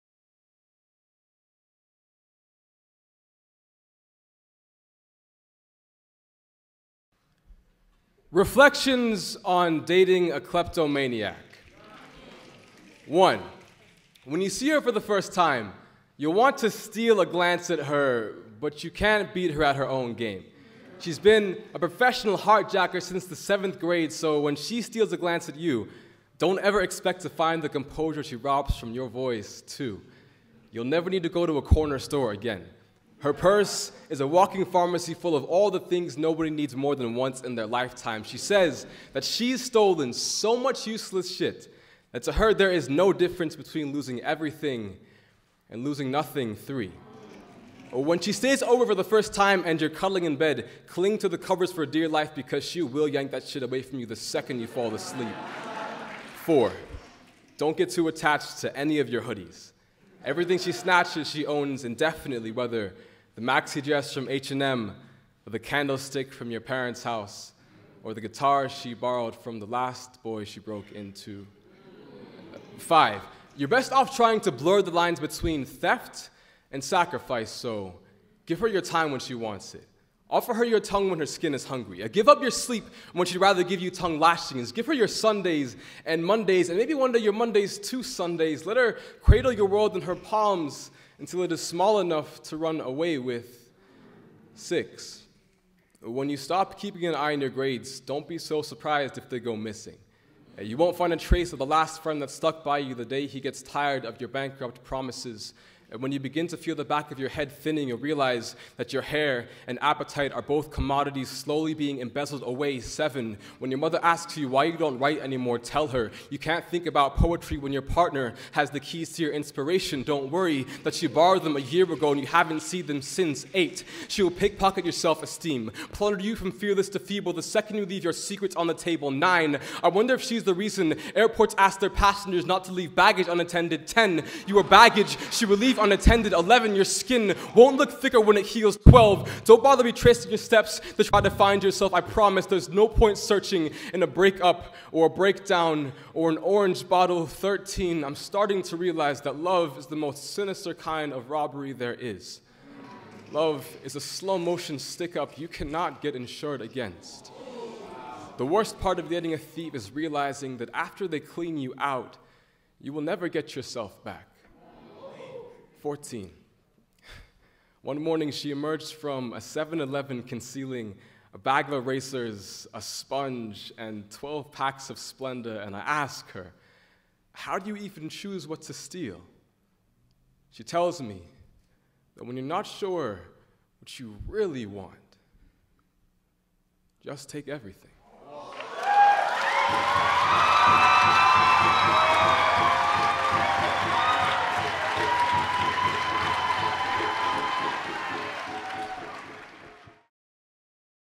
Tucker Bryant - Reflections on Dating a Kleptomaniac
Tucker Bryant, performing at CUPSI 2015 in Richmond, VA.